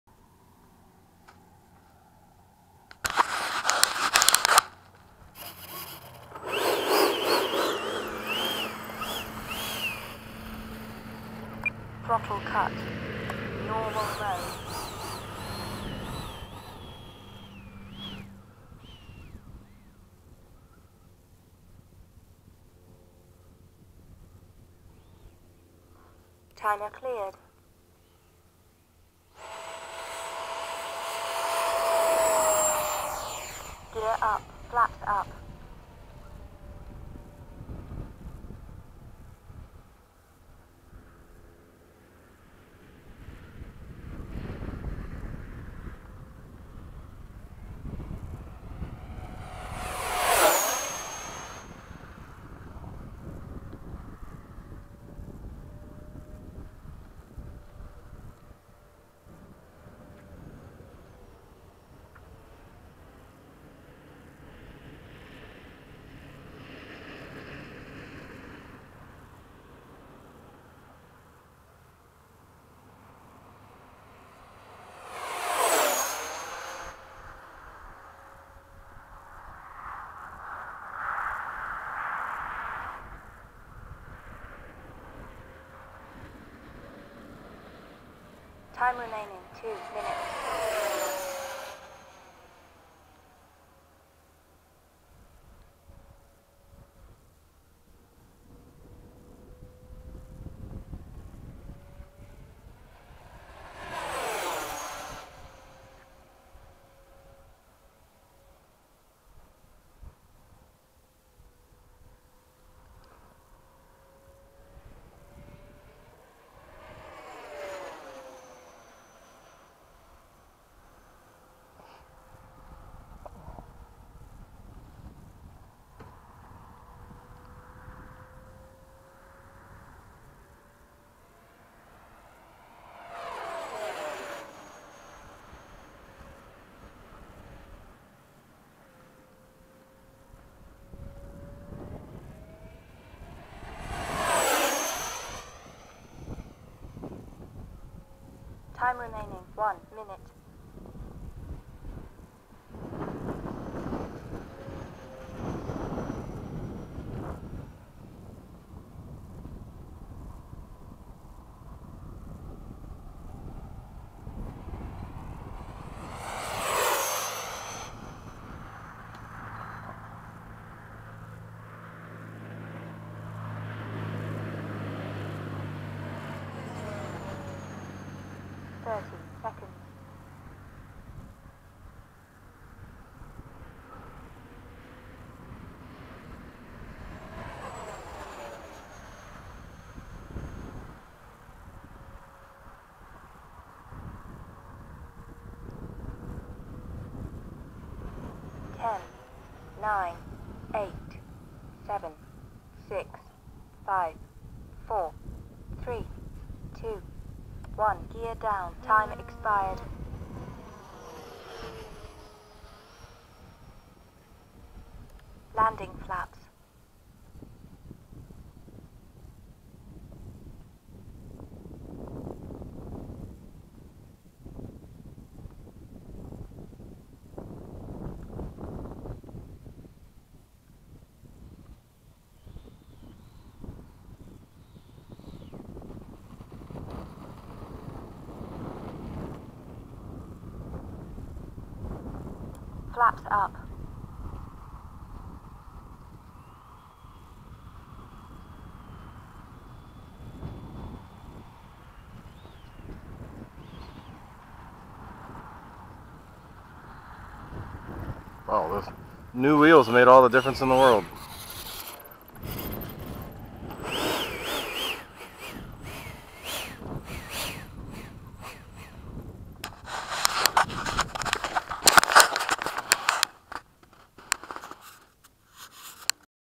Freewing 80mm F-5 mad man flight during sunset at LFE
Its old and ugly... but man is this thing a blast!!  Hopefully I will be able to get this bird redone during the down season...